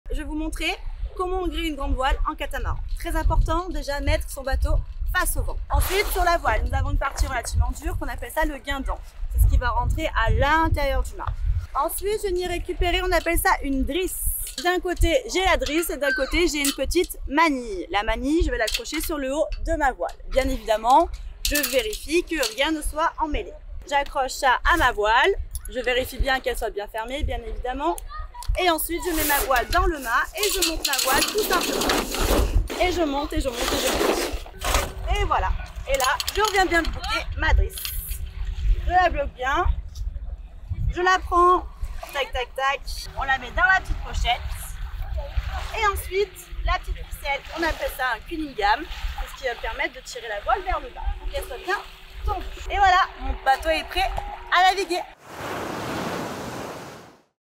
Ça tips pour moi – Catamaran : Comment hisser sa grand-voile
Tu adores le catamaran ? Notre monitrice te montre comment hisser ta grand-voile afin de pouvoir partir à l'aventure !

🔎Envie de faire du catamaran ?

⛵ Tu souhaites en savoir plus sur nos activités nautiques ?